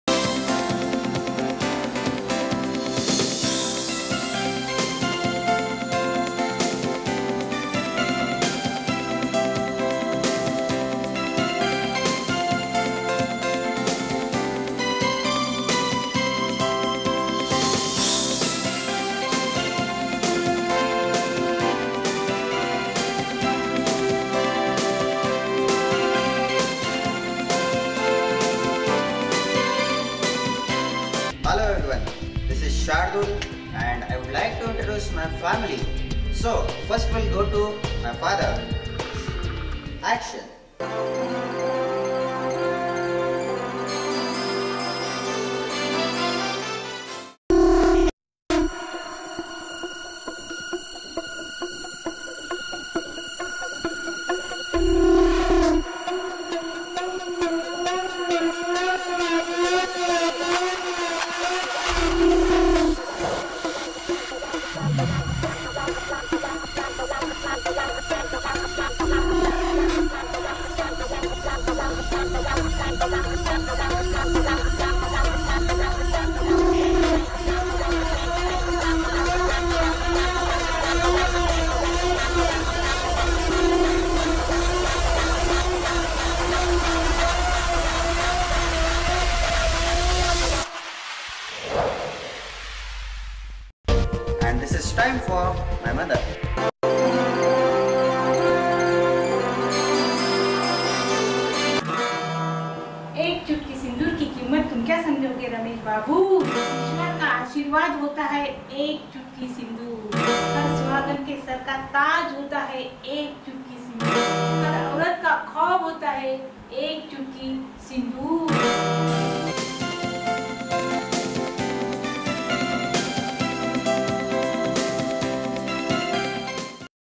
Shardul's Family's Got Talent
This video is selected as Most Favourite video (By Voting System).
Cast- Mr Omprakash Bonde (My Father), Mrs Rekha Bonde (My Mother)
Cinematography- Pushkar, Sagar, Amit, Andy and Shardul.
Directed & Edited by- Shardul Bonde.